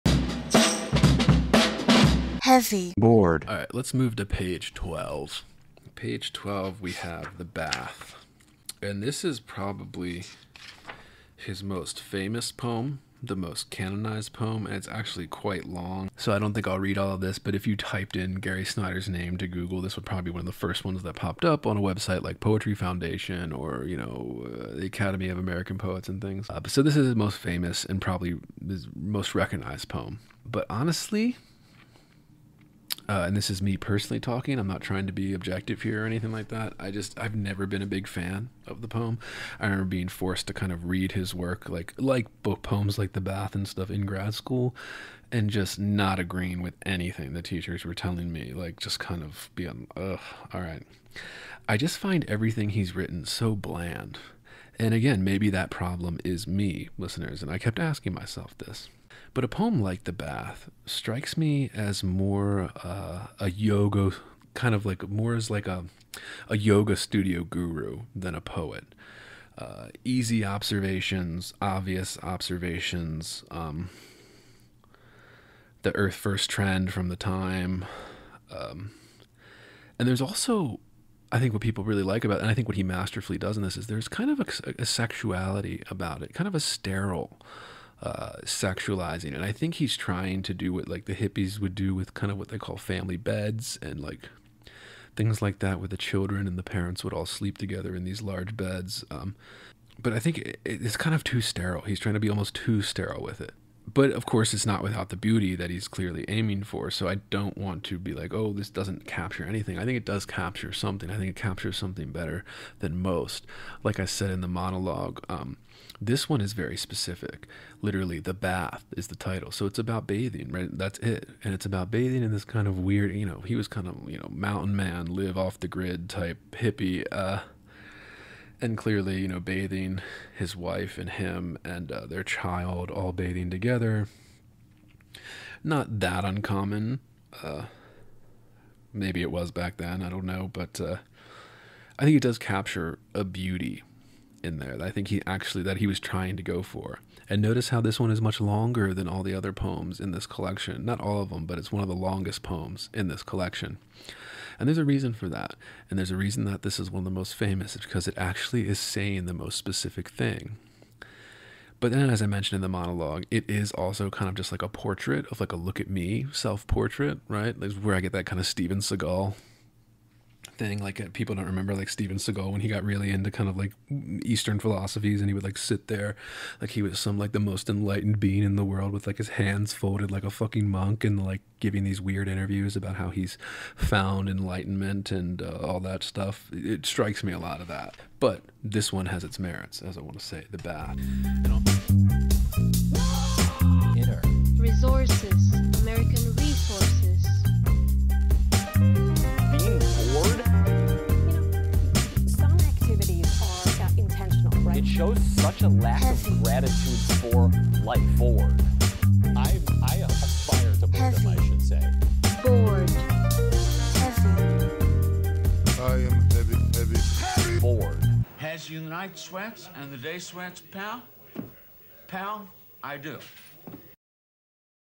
Gary Snyder's "The Bath"
Clip from Heavy Bored episode 39 Gary Snyder's Turtle Island

Andrew Wittstadt reviews Gary Snyder's prize-winning collection of poems, Turtle Island. Reading and going into the failures and triumphs of Snyder's poem, "The Bath"

Follow Heavy Bored!